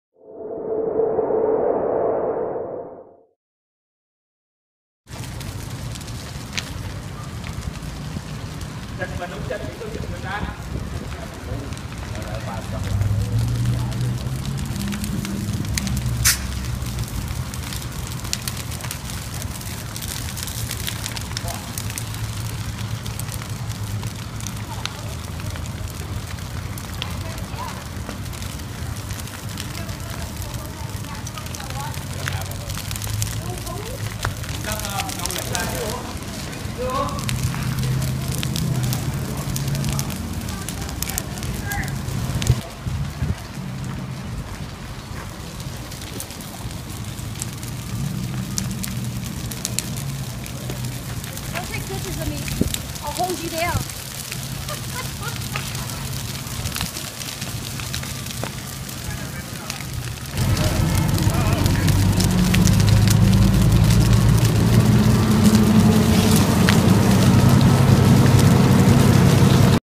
Brush fire in Sacramento parking lot
People attempt to put out a brush fire in a parking lot at Florin Road and Franklin Road on Wed., May 14, 2014. Firefighters arrived and extinguised the fire. No structures were damaged.